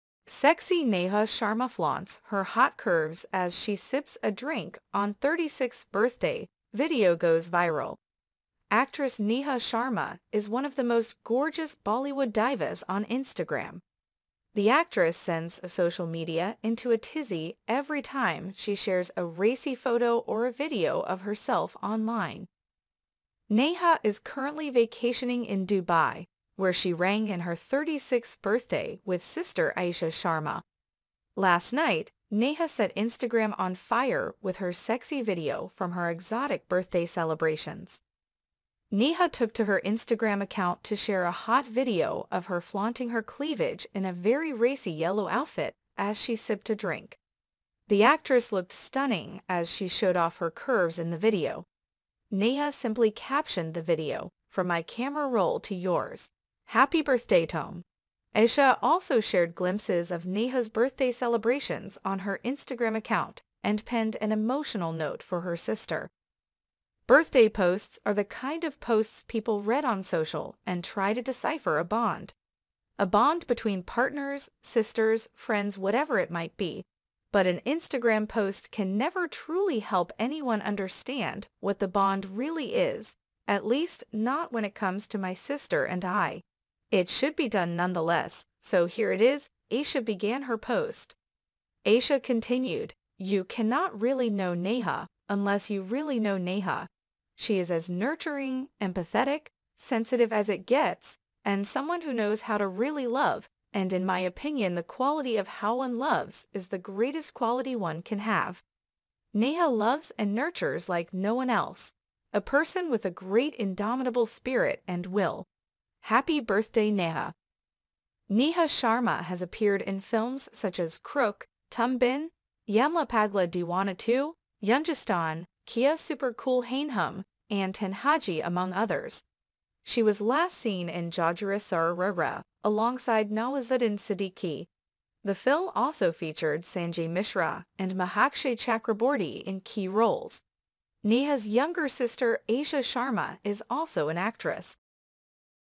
Sexy Neha Sharma Flaunts Her Hot Curves As She Sips A Drink On 36th Birthday, Video Goes Viral .
Bollywood actress Neha Sharma celebrated her 36th birthday in Dubai with her sister Aisha Sharma. In a viral video, she flaunted her curves while sipping a drink. Aisha also shared glimpses of her celebrations and praised Neha's nurturing, empathetic, and sensitive nature. Sharma has appeared in various films and is known for her indomitable spirit and will.